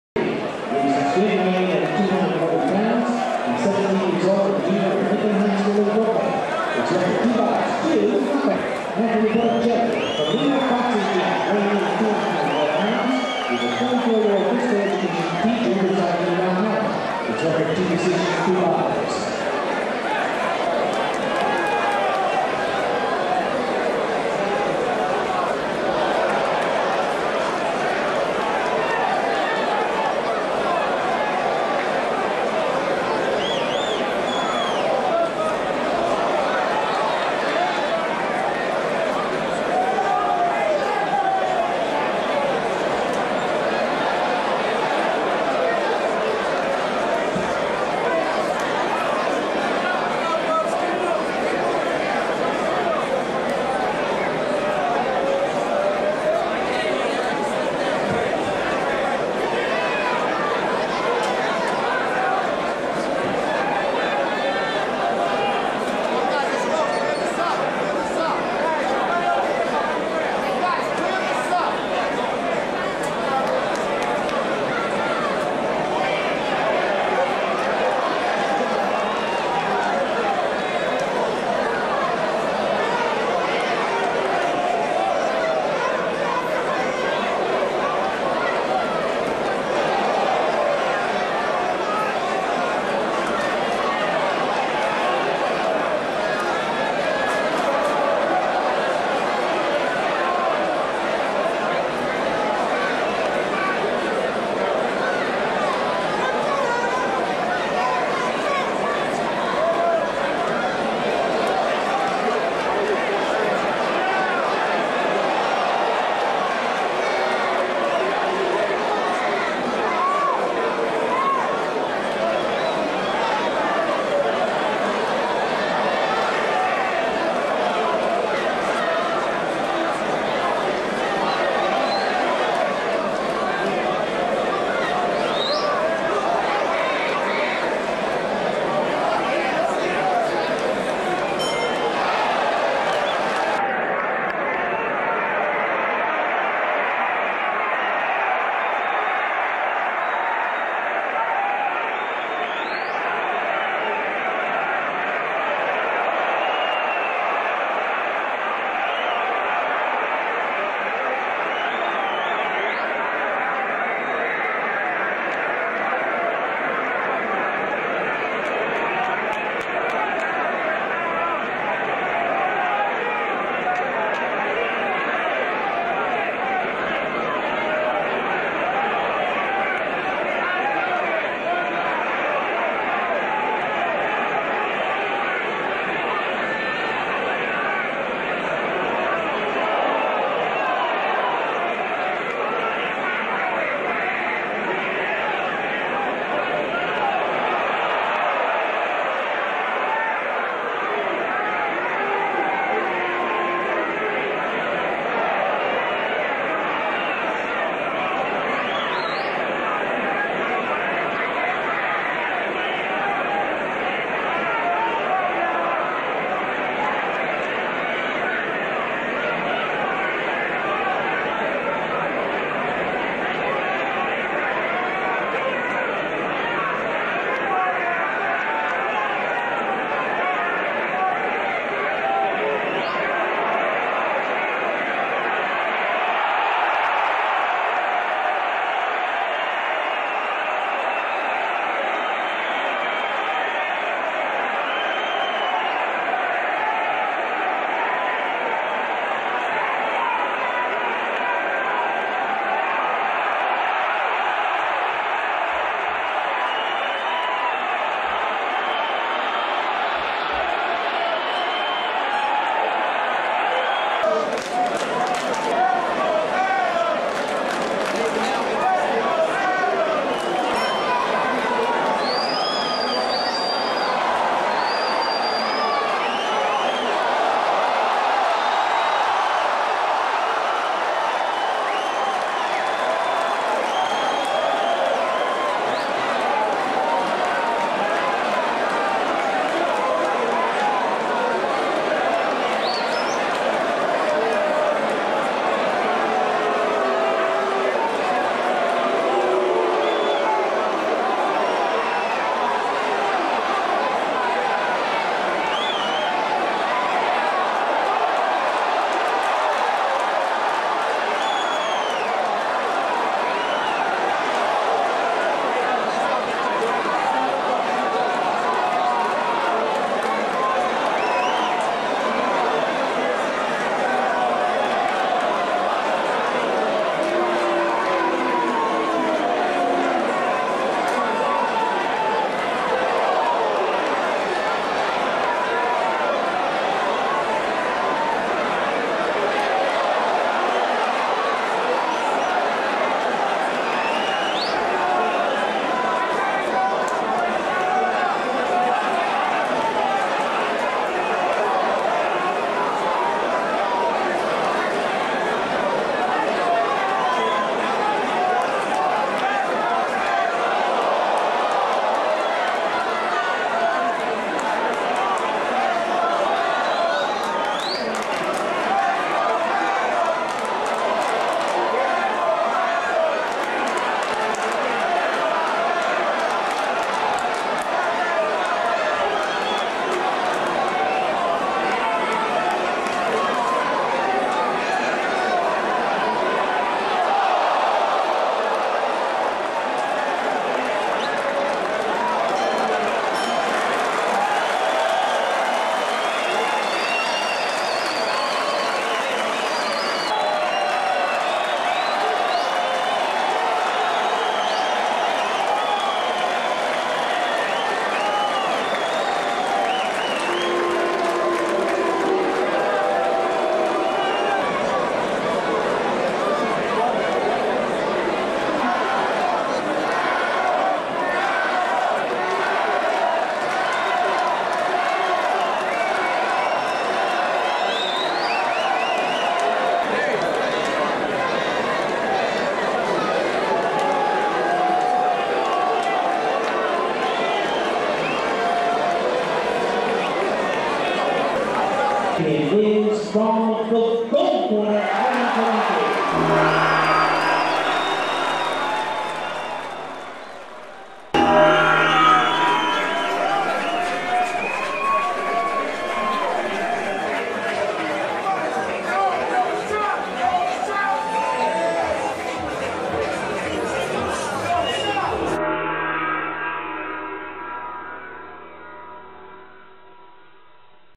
17 year old Adam Kownacki : Golden Gloves Champion
201+ lb. Championship @ Madison Square Garden 4/7/06 : Adam Kownacki. 17 year old. (record 2 KO) / Anthony Botticelli. 24 Year old. (record 2 decisions) : full fight (3 rounds)